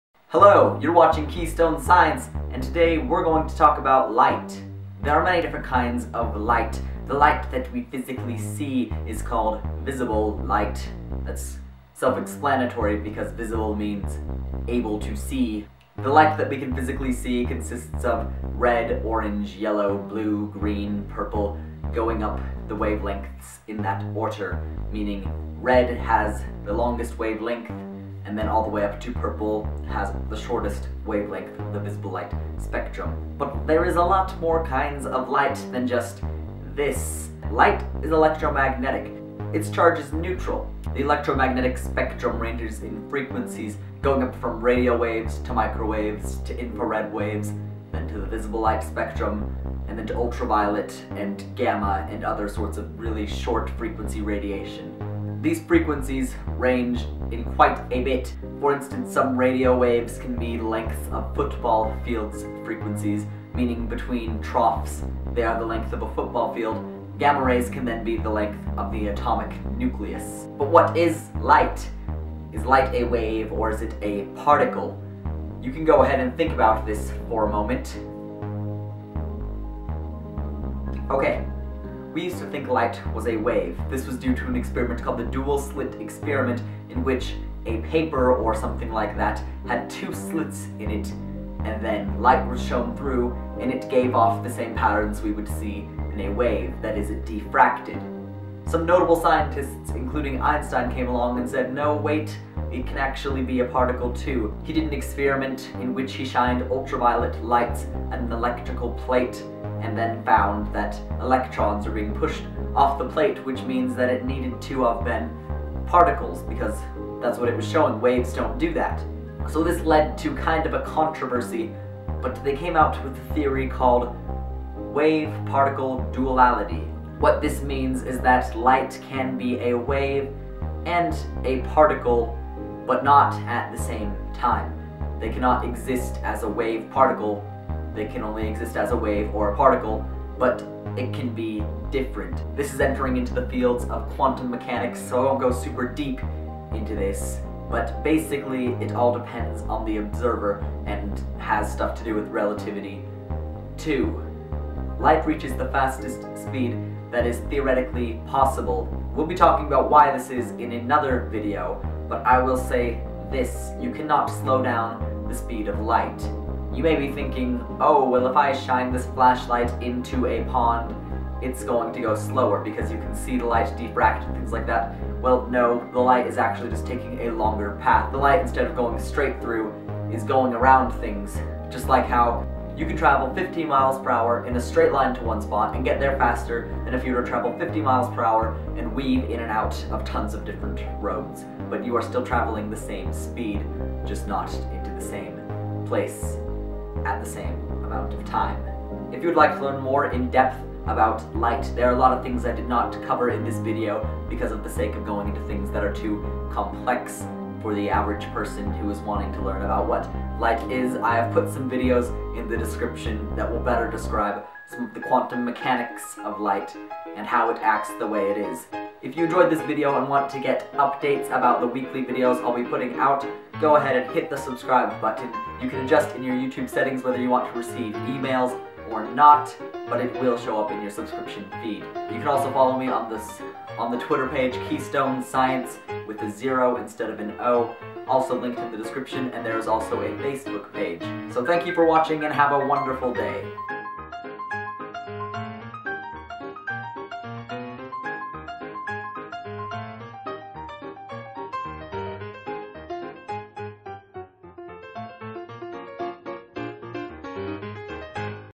What is Light?
Videos on Quantum Mechanics of light!
Please Leave bellow what you want to see next on the channel! i will be making new segments to teach science formulas you need help with in school and math concepts, Thanks for watching!